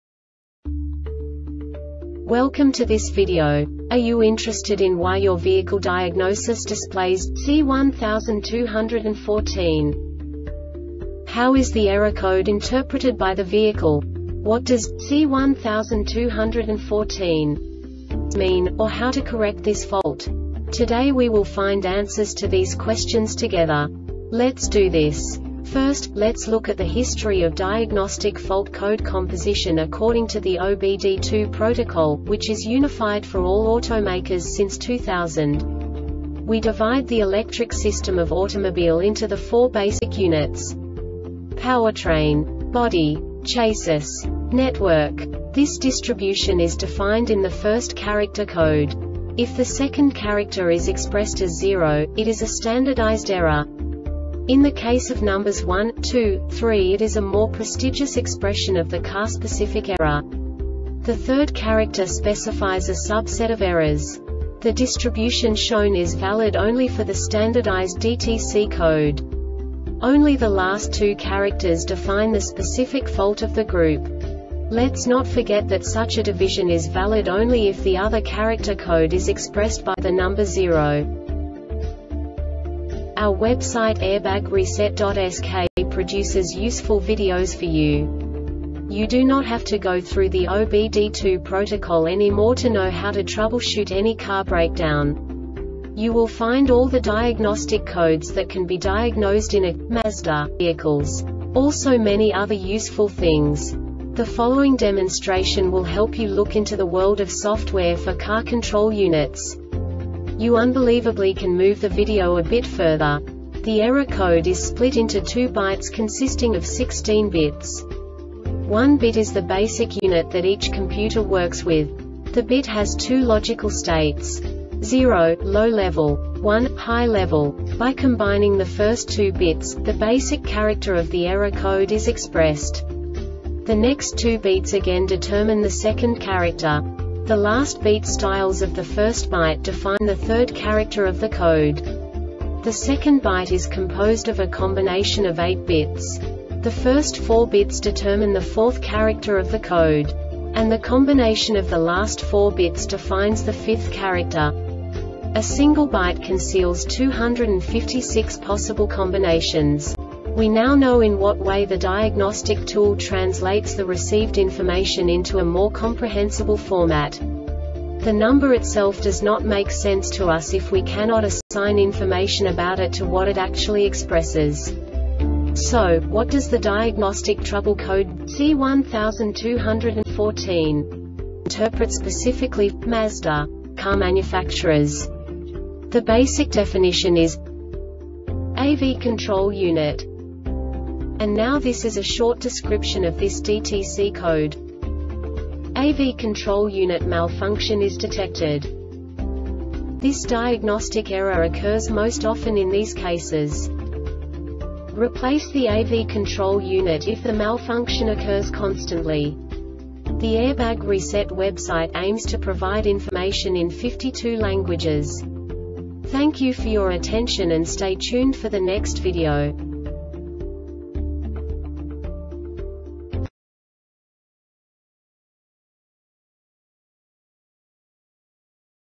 DTC Mazda C1214 Short Explanation
Contents:
0:21 Basic DTC analysis according to OBD2 protocol standard.
1:48 Insight into programming
2:58 A diagnostically understandable description of the Diagnostic Trouble Code
3:05 Basic error definition

Make:
Mazda
Code:
C1214
Definition:
AV Control Unit
Description:
AV control unit malfunction is detected.